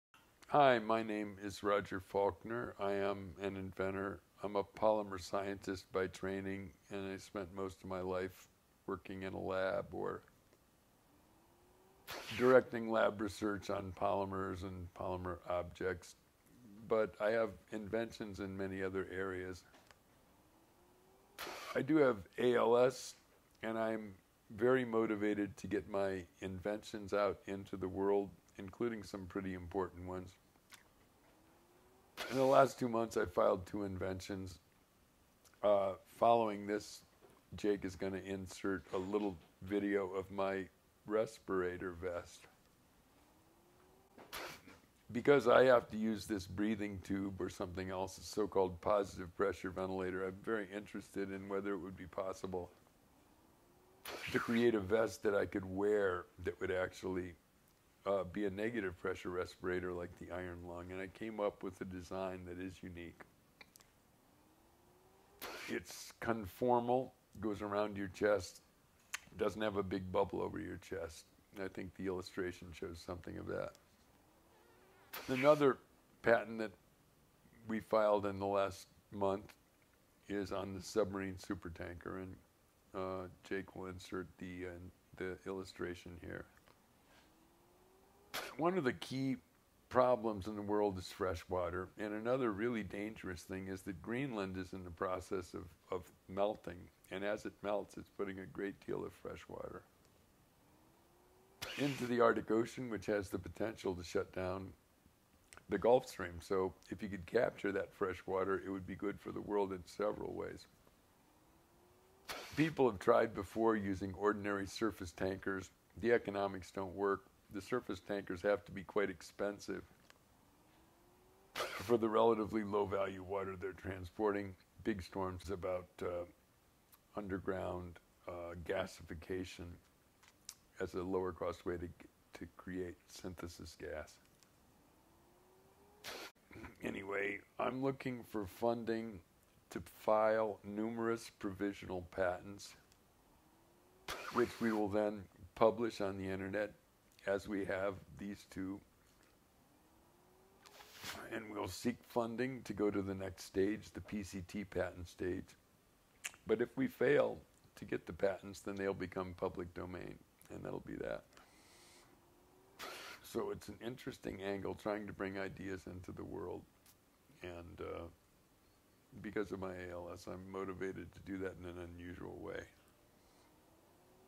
Rethink Technologies: GoFundMe Campaign
A video containing two models of our latest inventions we have been working on.